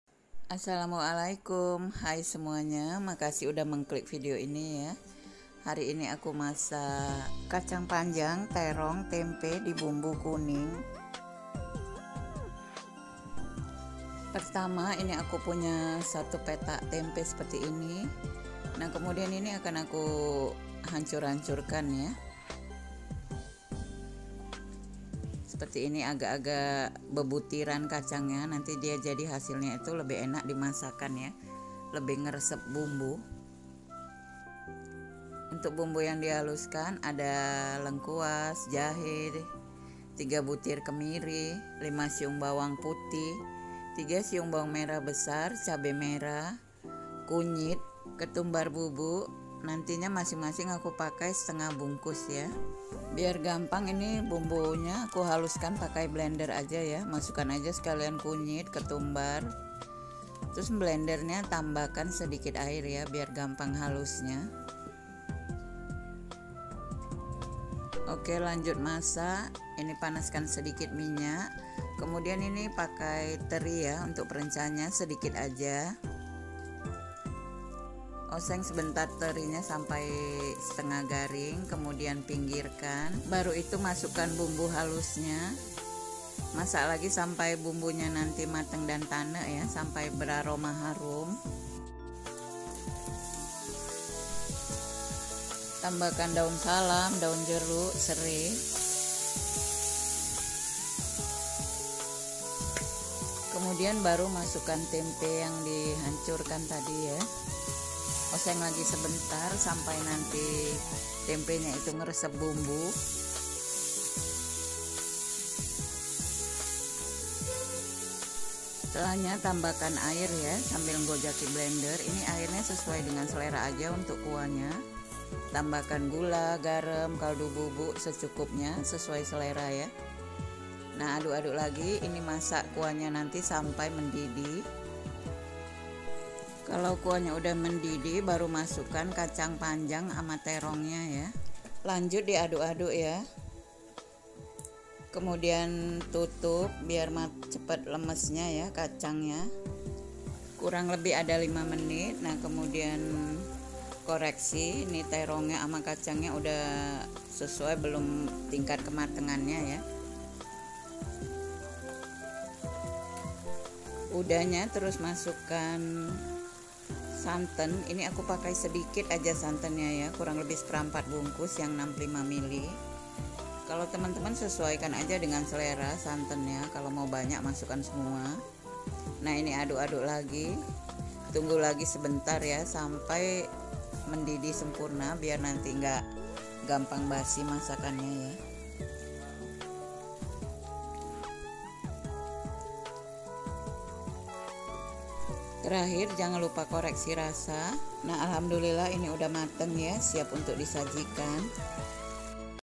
Gausah Lauk Lain Lagi, Ini aja Udah Enak Banget || Masak Pedes Bumbu Kuning !!
Jadi menu dan resep yang saya hadirkan hari adalah masak pedes bumbu kuning tempe terong teri, saya harap video saya buat dapat menambah pilahan menu makanan sehari-hari dirumah. ________________  ^_^ ^_^ ^_^